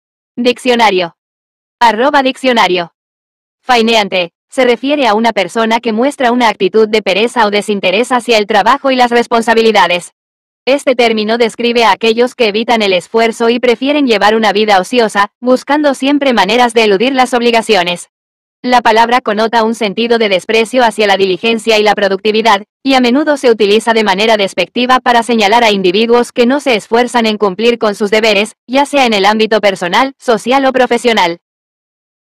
Fainéante Significado En Español
Fainéante: se refiere a una persona que muestra una actitud de pereza o desinterés hacia el trabajo y las responsabilidades. Este término describe a aquellos que evitan el esfuerzo y prefieren llevar una vida ociosa, buscando siempre maneras de eludir las obligaciones. La palabra connota un sentido de desprecio hacia la diligencia y la productividad, y a menudo se utiliza de manera despectiva para señalar a individuos que no se esfuerzan en cumplir con sus deberes, ya sea en el ámbito personal, social o profesional.